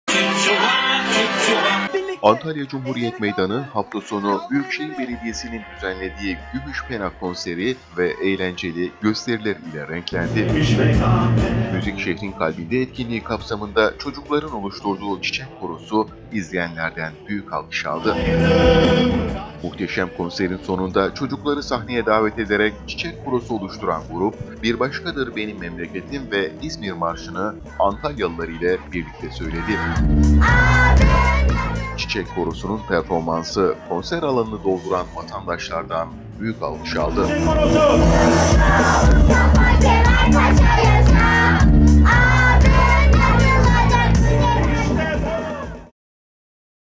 Çocuklardan ‘Çiçek korosu’ -ANTALYA
Antalya Cumhuriyet Meydanı hafta sonu Büyükşehir Belediyesi’nin düzenlediği Gümüş Pena konseri ve eğlenceli gösteriler ile renklendi. Müzik Şehrin Kalbinde etkinliği kapsamında çocukların oluşturduğu çiçek korosu da izleyenlerden büyük alkış aldı.